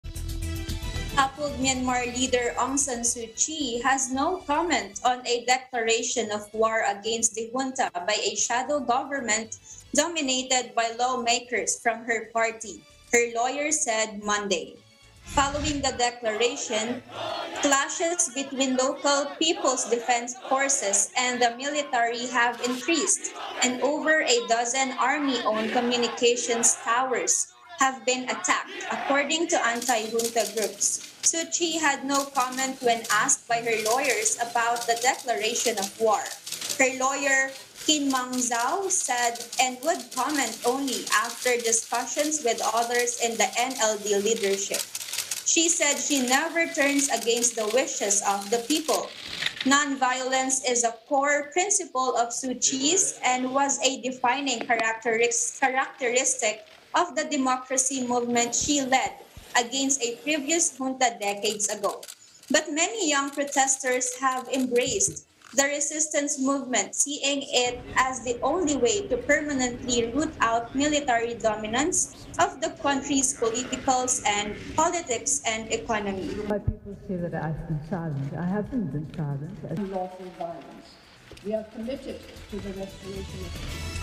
Myanmar's Suu Kyi has 'no comment' on call for war against junta
Toppled Myanmar leader Aung San Suu Kyi has "no comment" on a declaration of war against the junta by a shadow government dominated by lawmakers from her party, her lawyer said.